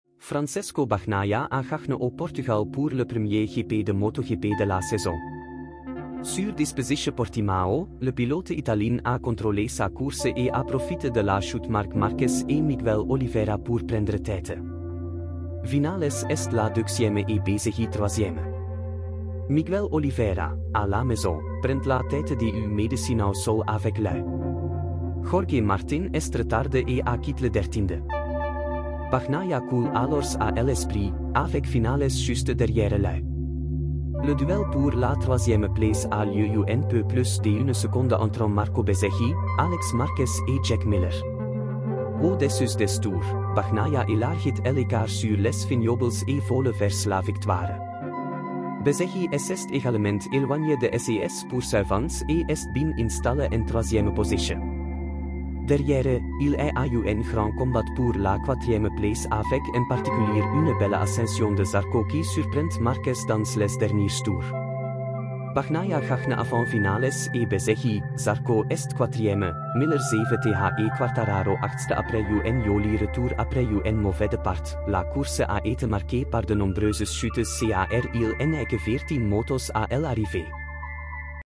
MotoGP Portugal : Bagnaia s'impose devant Vinales, Quartararo 8e, Marquez chute
Francesco Bagnaia a gagné au Portugal pour le premier GP de MotoGP de la saison. Sur disposition Portimao, le pilote italien a contrôlé sa course et a profité de la chute Marc Marquez et Miguel Oliveira pour prendre tête. Vinales est la deuxième et Beezecchi troisième. Miguel Oliveira, à la maison, prend la tête du médecinau sol avec lui. Jorge Martin est retardé et a quitté le 13e. Bagnaia coule alors à l'esprit, avec Vinales juste derrière lui. Le duel pour la troisième place a lieu un peu plus d'une seconde entre Marco Bezzecchi, Alex Marquez et Jack Miller. Au-dessus des tours, Bagnaia élargit l'écart sur les vignobles et vole vers la victoire. Bezzecchi s'est également éloigné de ses poursuivants et est bien installé en troisième position. Derrière, il y a un grand combat pour la quatrième place avec en particulier une belle ascension de Zarco qui surprend Marquez dans les derniers tours. Bagnaia gagné avant Vinales et Bezzecchi.zarco est quatrième, Miller 7th et Quartararo 8e après un joli retour après un mauvais départ.La course a été marquée par de nombreuses chutes car il n'y que 14 motos à l'arrivée.